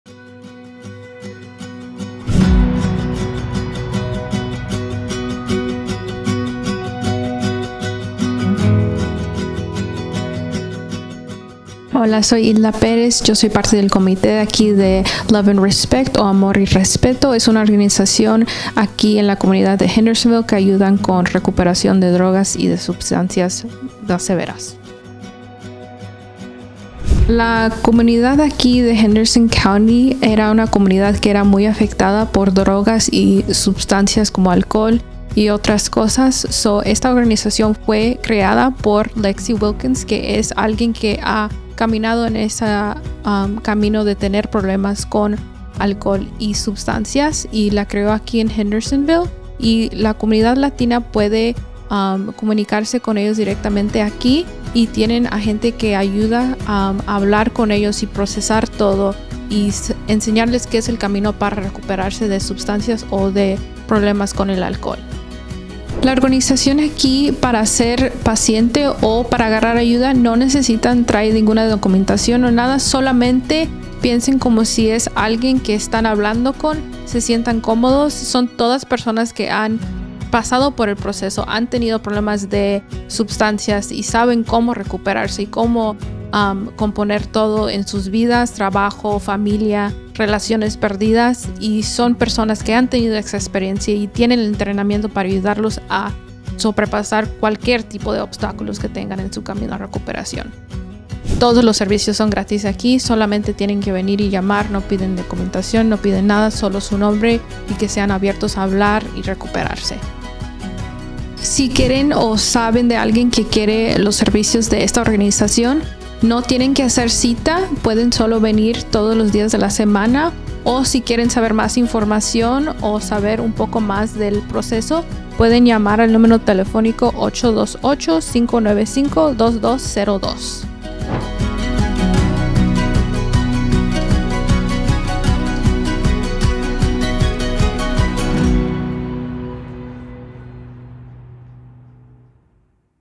😊 Te presentamos a Love and Respect ❤️
Love and Respect ayuda a individuos a recuperarse de abuso de sustancias, problemas de salud mental y reingreso tras encarcelamiento mediante un auténtico Apoyo entre Pares. Conversamos con Hilda Pérez, quien forma parte de L&R.
Love and Respect helps individuals recover from substance abuse, mental health issues, and re-entry after incarceration through authentic Peer Support. We talked with Hilda Pérez, who is a member of L&R.